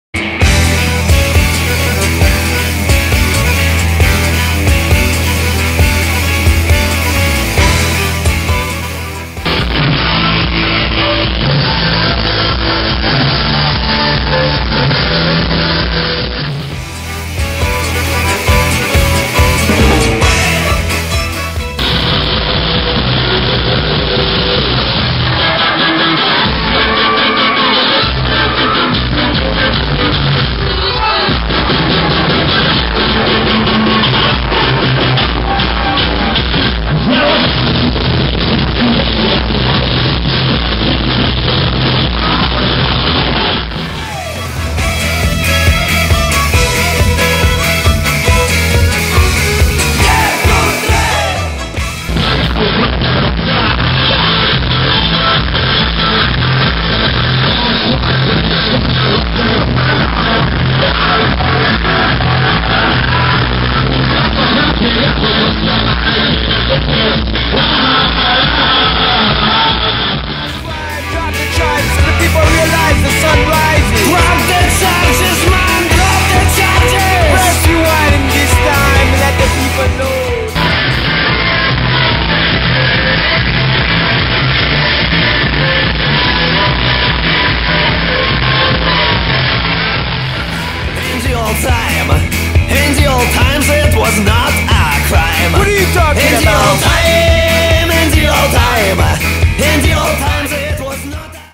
Gogol Bordello Video and Photos - the Newport Columbus, OH
Video clips and photos from the Gogol Bordello show at the Newport in Columbus, OH on October 27, 2009